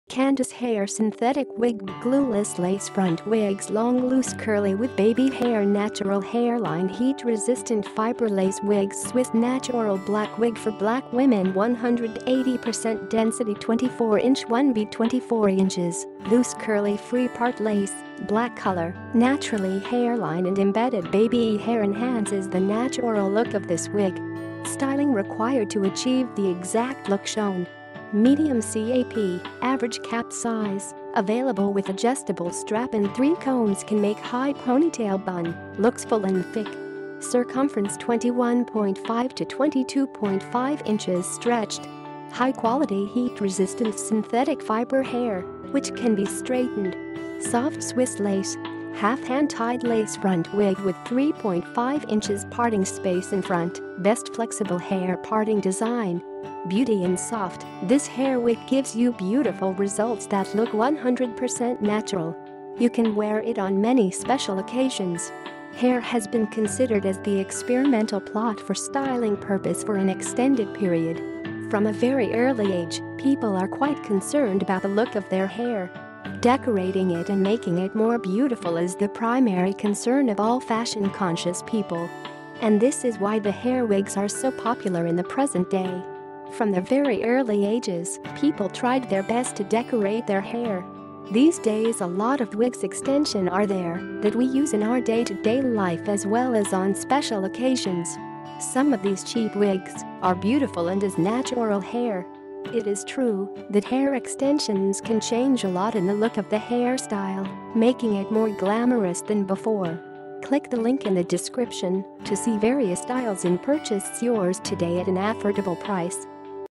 Candice Hair Synthetic Wig Glueless Lace Front Wigs Long Loose Curly with Baby Hair Natural
Twenty-four inches, Loose Curly, Free Part Lace, Black Color, naturally hairline and embedded baby-hair enhances the natural look of this wig. Styling required to achieve the exact look shown.
Medium cap(Average cap size) available with adjustable strap and three combs can make high ponytail bun, looks full and thick. Circumference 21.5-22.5 inches stretched
High quality heat resistant synthetic fiber hair, which can be straightened
Soft Swiss Lace, Half Hand Tied Lace Front Wig with 3.5 inches Parting Space in Front, Best Flexible Hair Parting Design
BEAUTY & SOFT - This Hair Wigs give you beautiful results that look 100% natural. You can wear it on many special occasions.